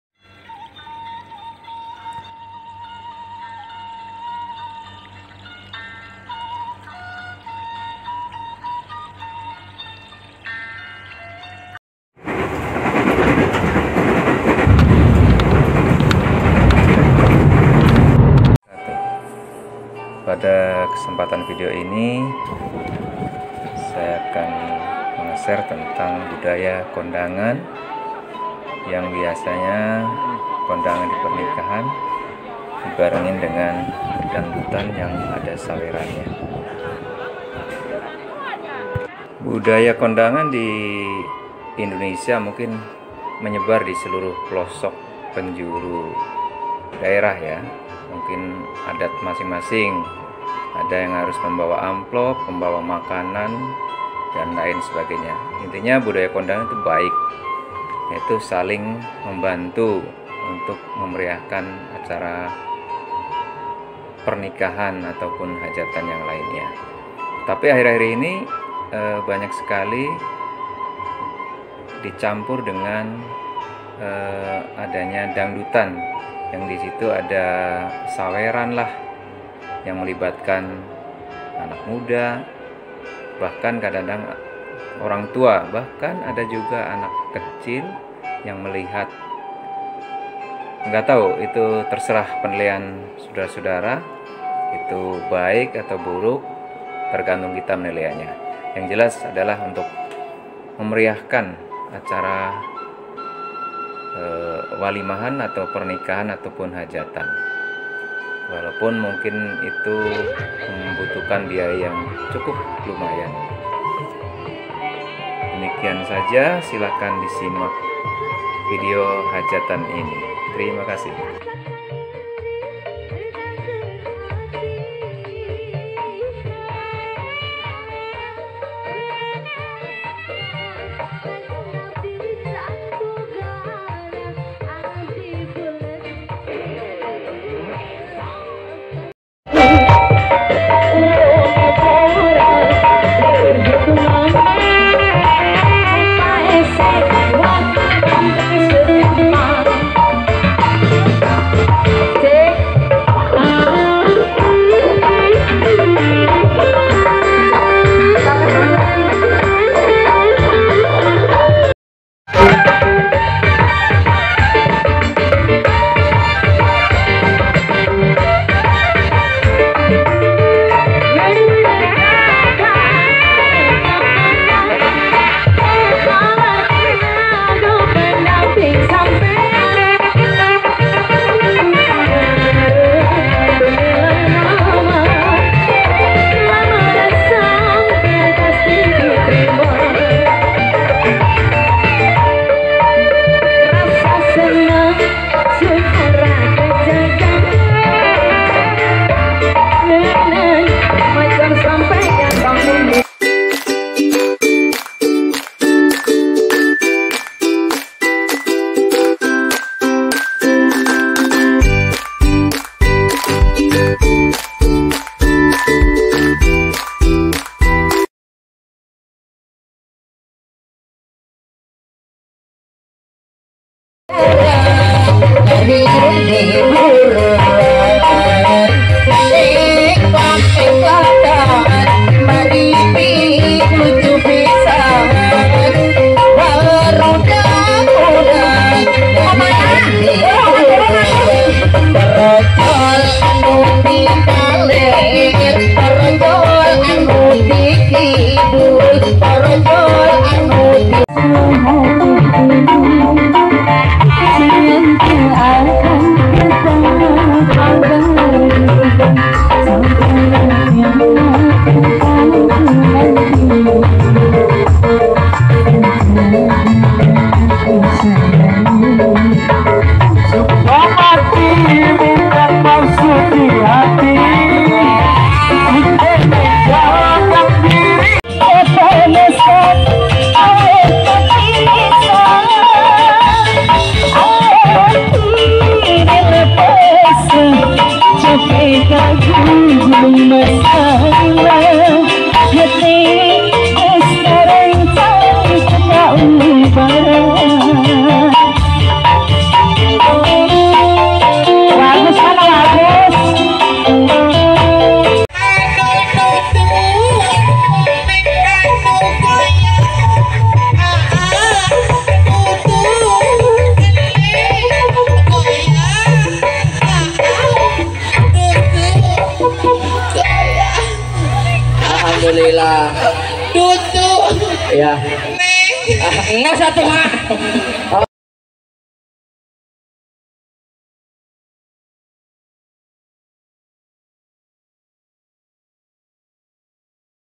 Budaya Kondangan dan Saweran Dangdut Yisscompetition # saweran biduan dangdut
Budaya Kondangan di Indonesia sudah menjadi tradisi sejak dulu,baik di perkotaan apalagi di pedesaan.
Namun dalam perkembangannya budaya kondangan kini dibarengi dengan budaya saweran dangdutan,,yang mungkin dulu ada pertunjukan lengger,ronggeng,sekarang tergerus berganti dengan pertunjukan dangdutan,orgen tunggal.,dan lainnya.

Terima kasih sudah mampir di channel saya

Budaya Kondangan